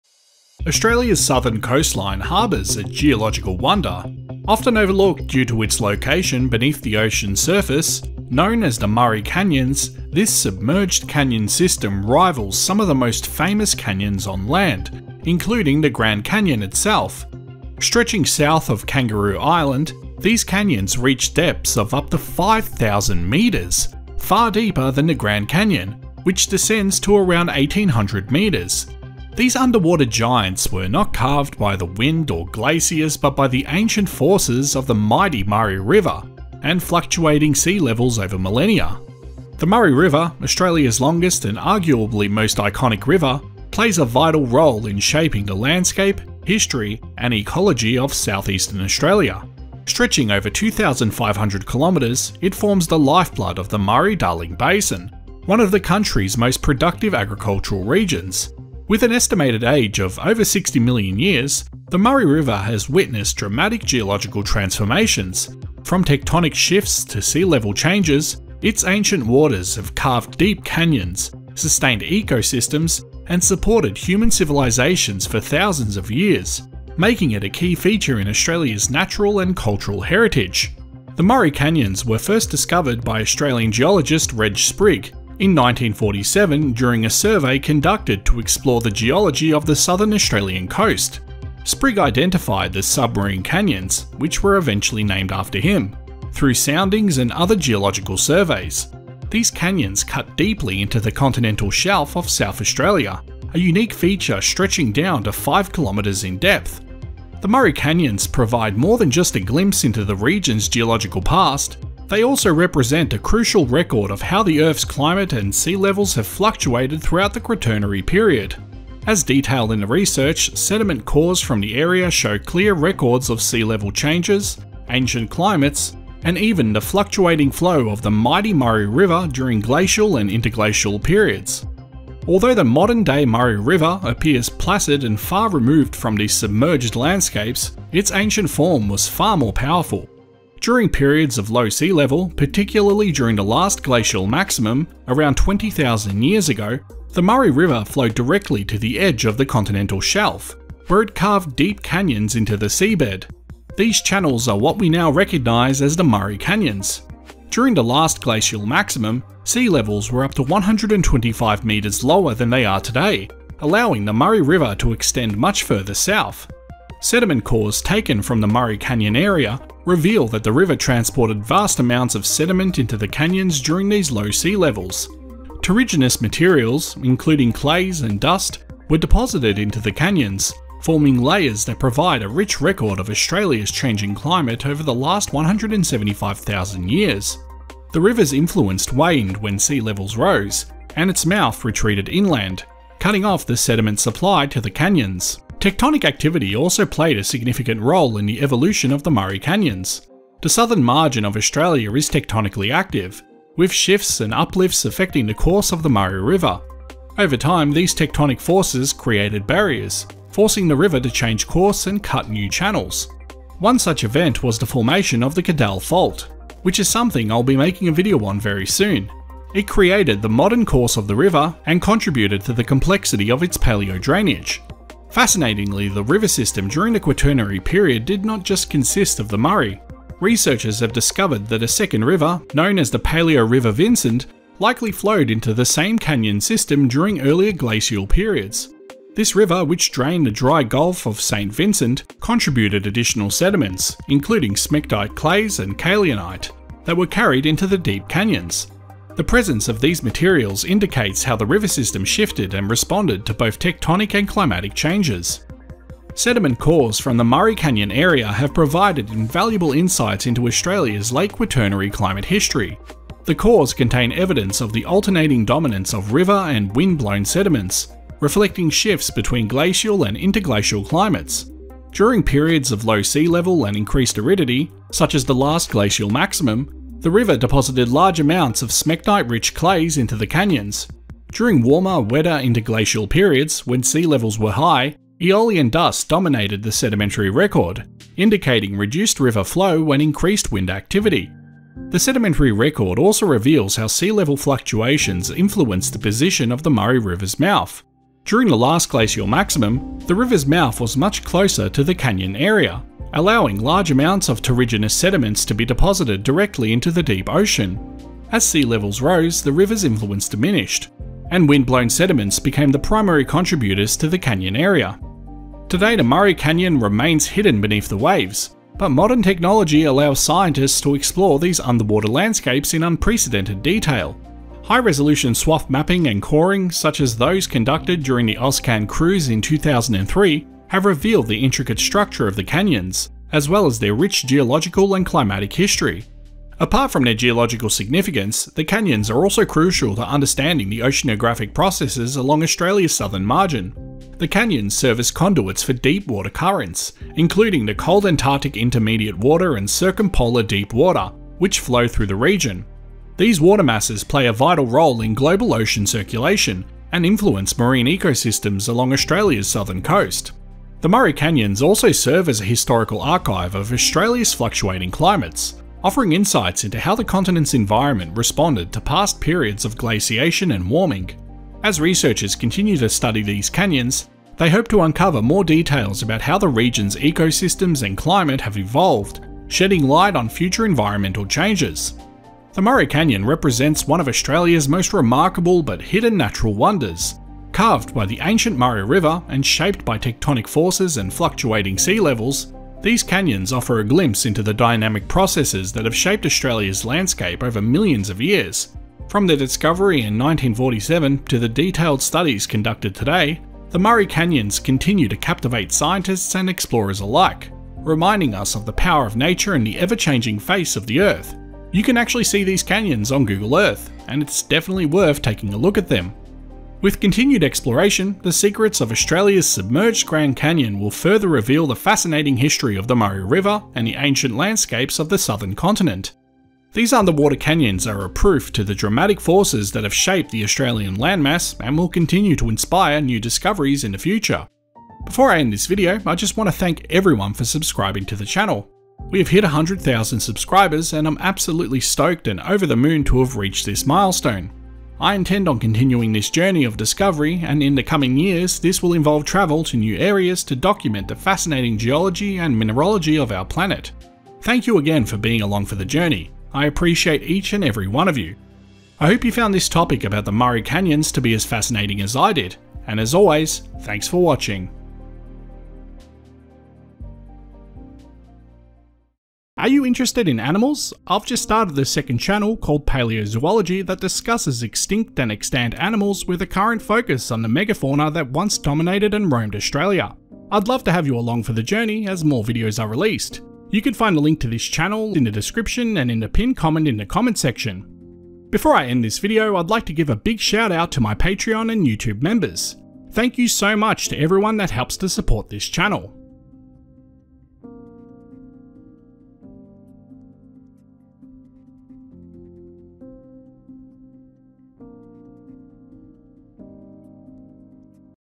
Australia's Hidden Grand Canyon
Explore the incredible hidden wonder of the Murray Canyon, an expansive underwater canyon system located off the coast of South Australia. Carved over millennia by ancient rivers and geological forces, this submerged canyon reaches depths of up to 5,000 meters—more than twice as deep as the Grand Canyon. In this video, we dive into the history, discovery, and significance of the Murray Canyons, a landscape that holds critical clues about the ancient tectonic shifts, sea level changes, and river systems that shaped Australia's southern coastline.

Discover how the Murray River, tectonic activity, and shifting sea levels created this natural marvel beneath the waves. We’ll also explore the sediment cores that reveal a detailed history of climate change, sea level fluctuations, and even the presence of ancient rivers like the Palaeo-River Vincent. Join me on this journey through Australia's submerged geological heritage!